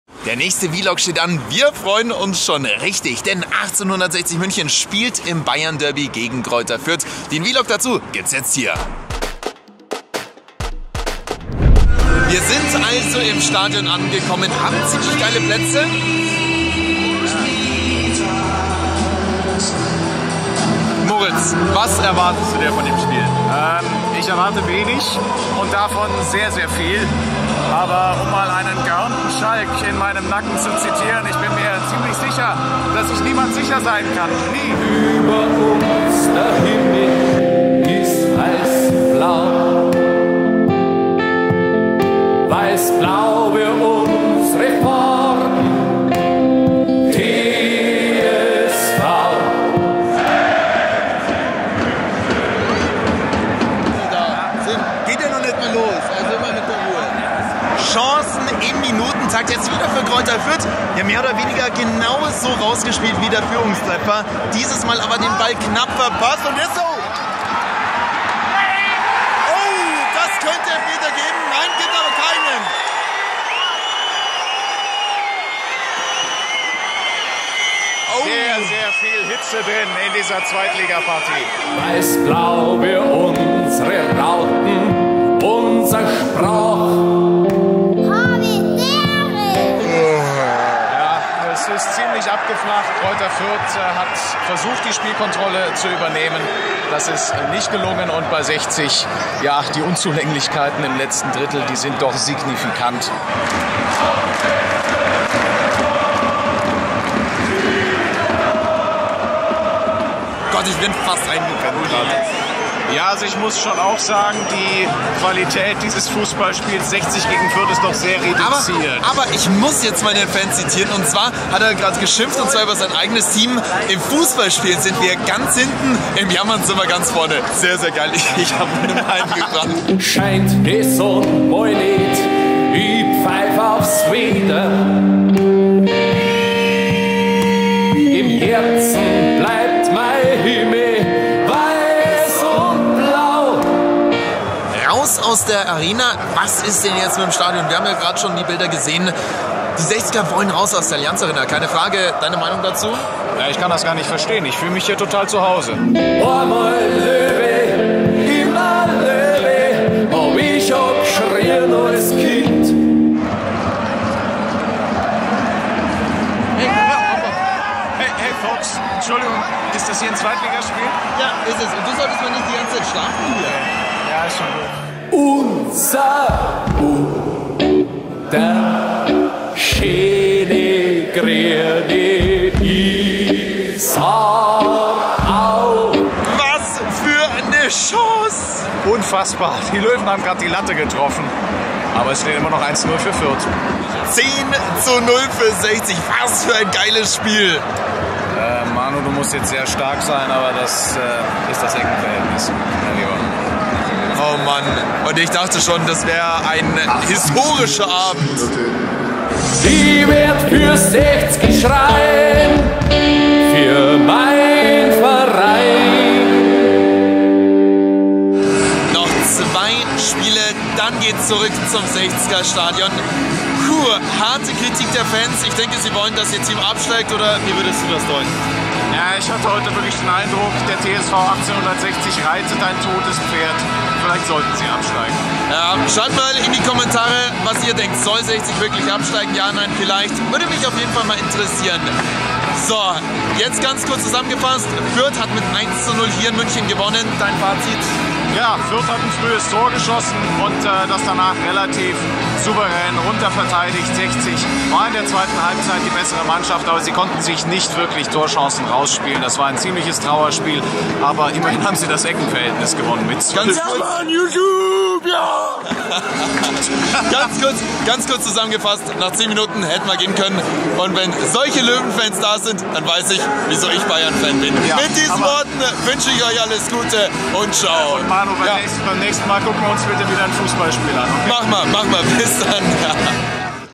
1860 München im Abstiegskampf! | VLOG 2. Fußball-Bundesliga | vs. Greuther Fürth
Das Abstiegs-Gespenst ist zu spüren. 1860 München kämpft in der 2. Fußball-Bundesliga gegen den Abstieg. Im Bayern-Derby gegen Greuther Fürth sind wir in der Allianz-Arena gewesen. Viel Spaß beim Vlog!

Das Equipment:
* Meine News-Kamera
* Meine Vlog-Kamera
* Mein Mikrofon
* Mein Licht
* Meine Schnitt-Software
* Mein Macbook
* Mein Mac